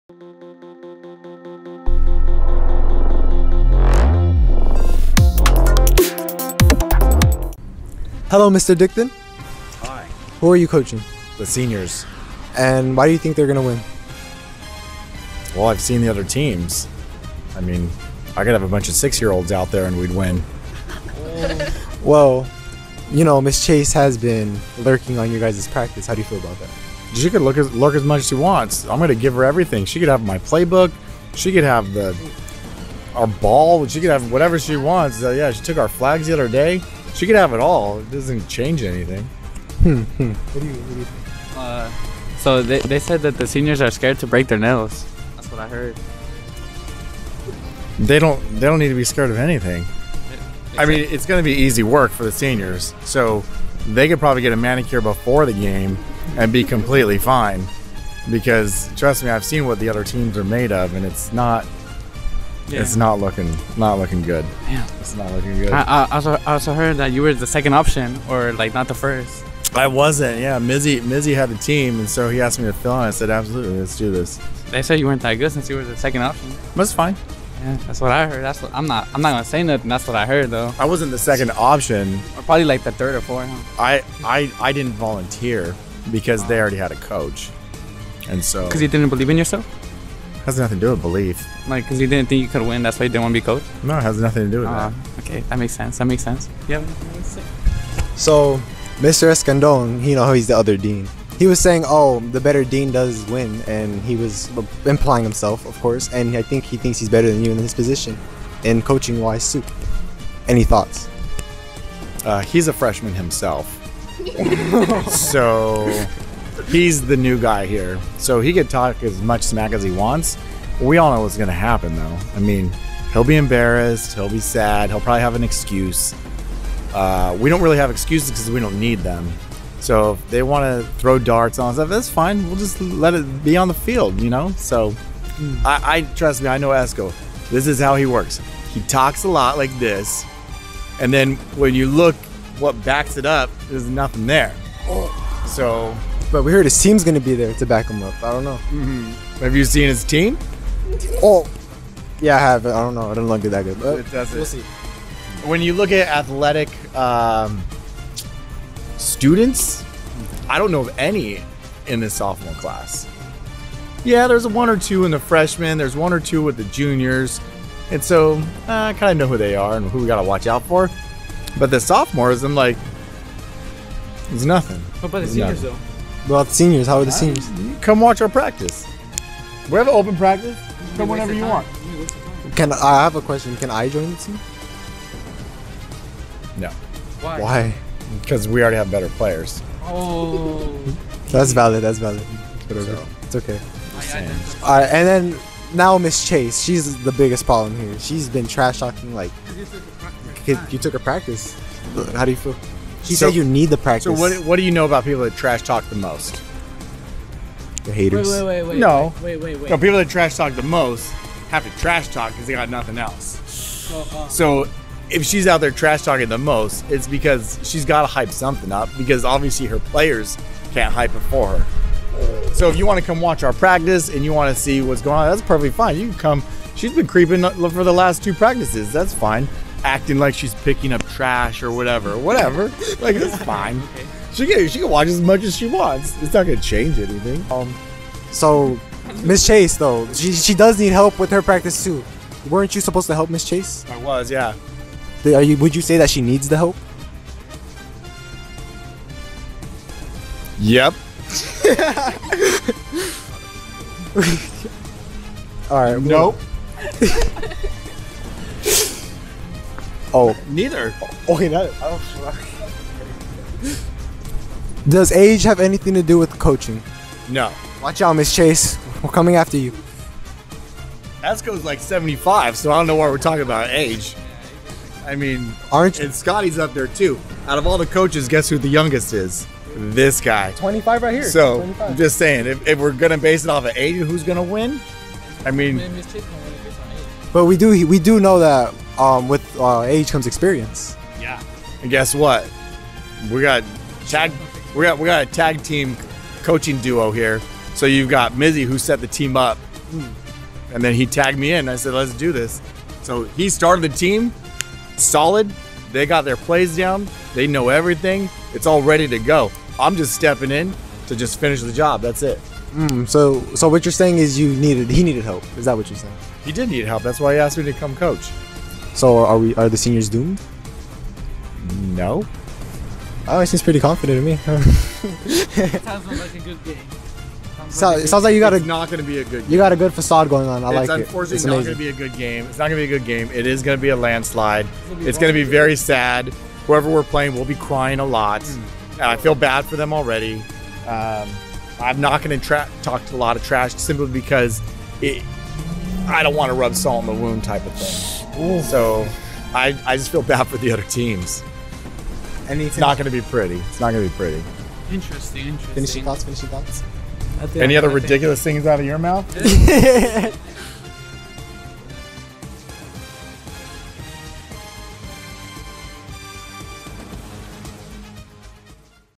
Powderpuff: Seniors-Dikdan
A interview with the Senior powderpuff coach, Mr. Dikdan.
Producers: Ethan, Sofia, Manny
 Editor: Gabe